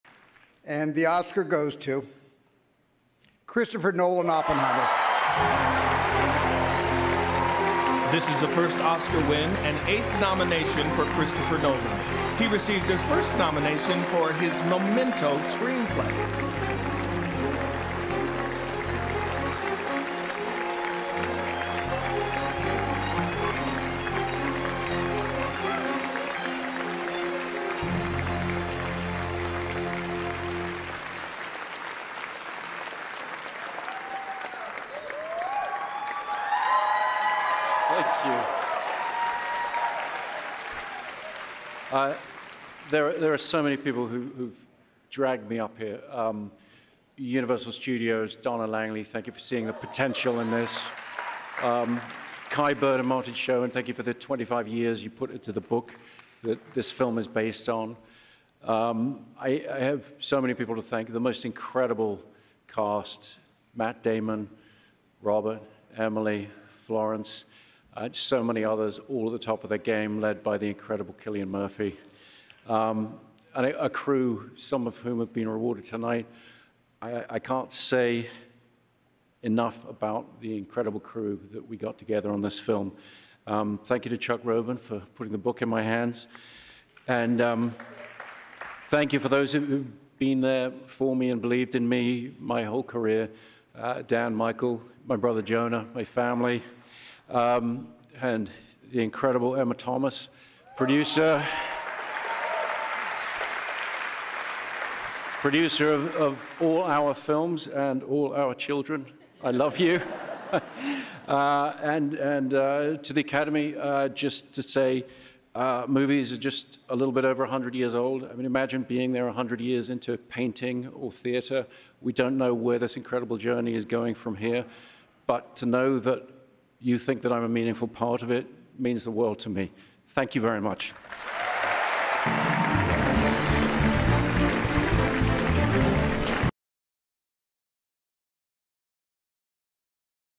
Oscar 2024: Christopher Nolan gana Mejor director por Oppenheimer
Tras ser nominado en ocho ocasiones, ChristopherNolan finalmente gana Mejor director por Oppenheimer.